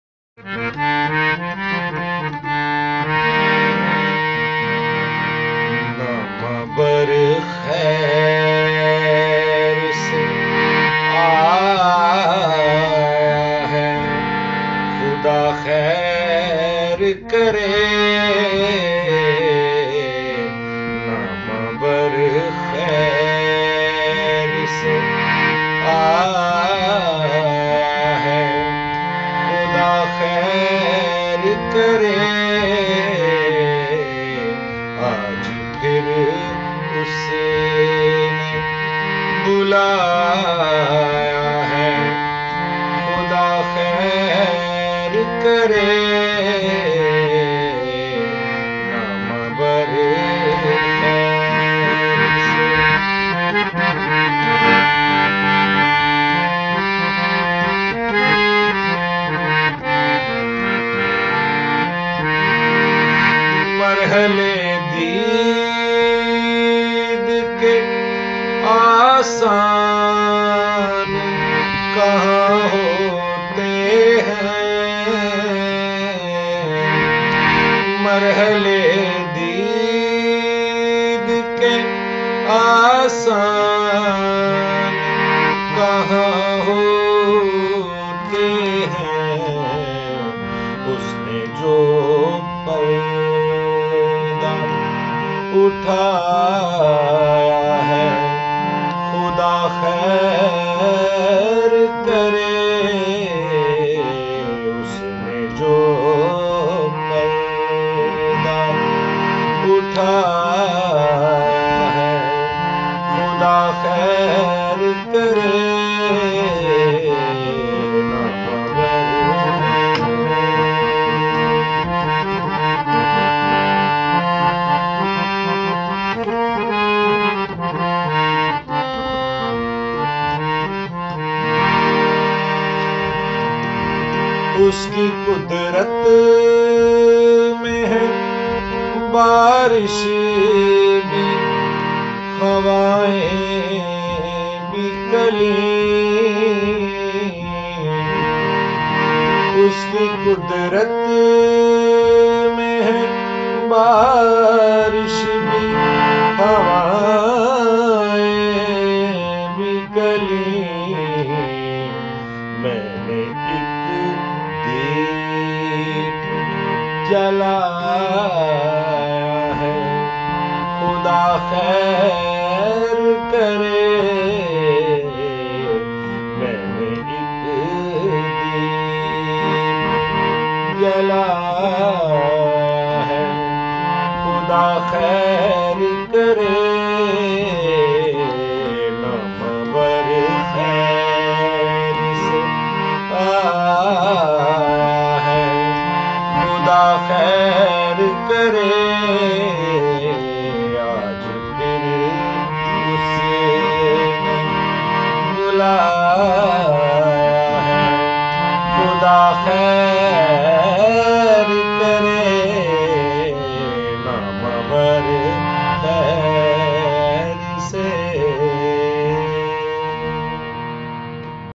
A very beautiful gazal in classical style (asif butt)
classical music and Urdu poetry having a very close and important relation b/w them.In this video asif butt shows this relation in very beautiful manner.Listen it and enjoy the beautiful trance of classical music.